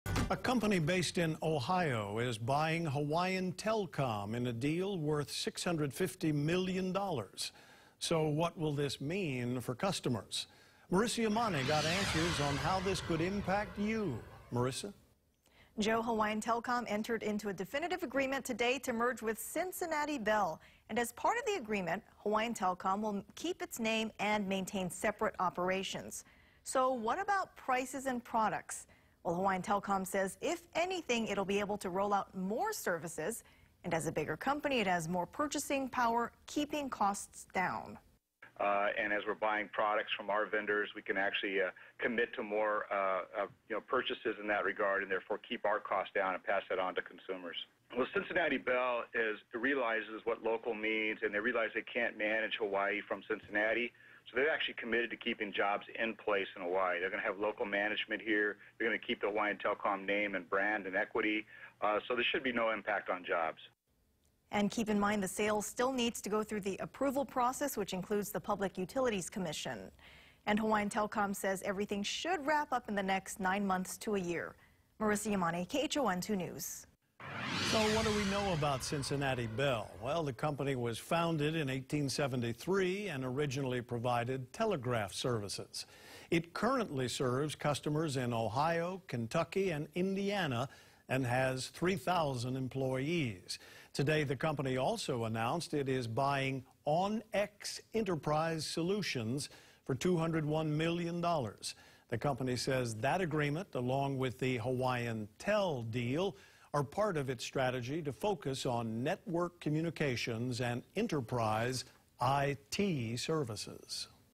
Hawaiian Telcom to merge with Ohio company in $650 million deal
Both companies will continue to maintain their separate local brand identities and operations, and no jobs in Hawaii should be affected.